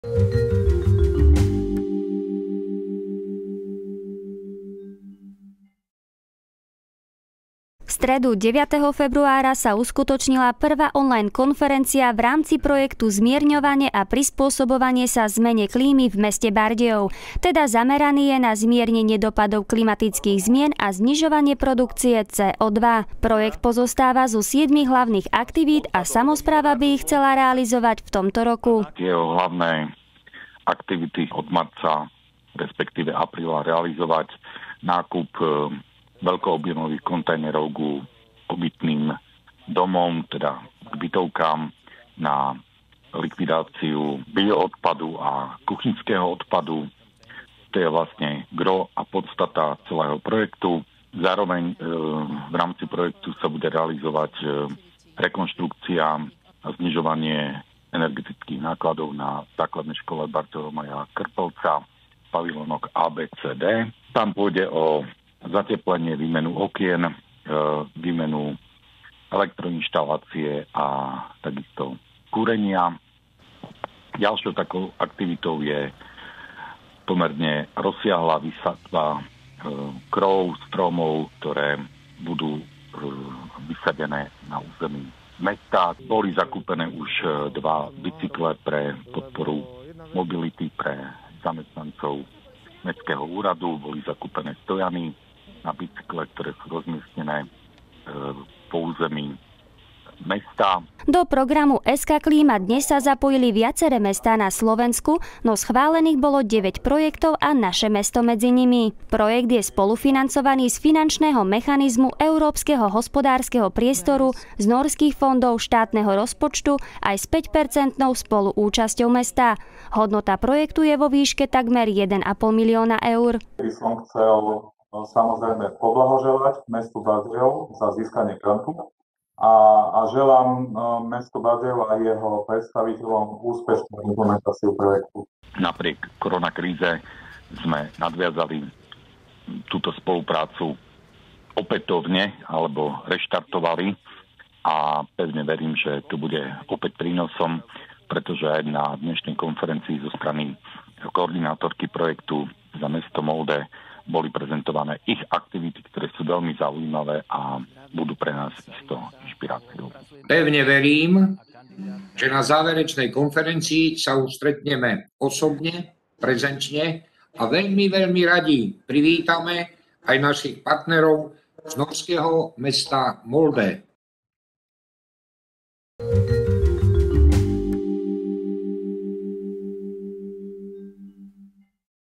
Prvá konferencia o zmierňovaní klímy
V stredu, 9. februára sa uskutočnila prvá online konferencia v rámci projektu Zmierňovanie a prispôsobovanie sa zmene klímy v meste Bardejov. Projekt je schválený takmer vo výške 1,5 milióna eur a predstavili aj sedem hlavných aktivít, ktoré plánuje samospráva mesta realizovať v tomto roku.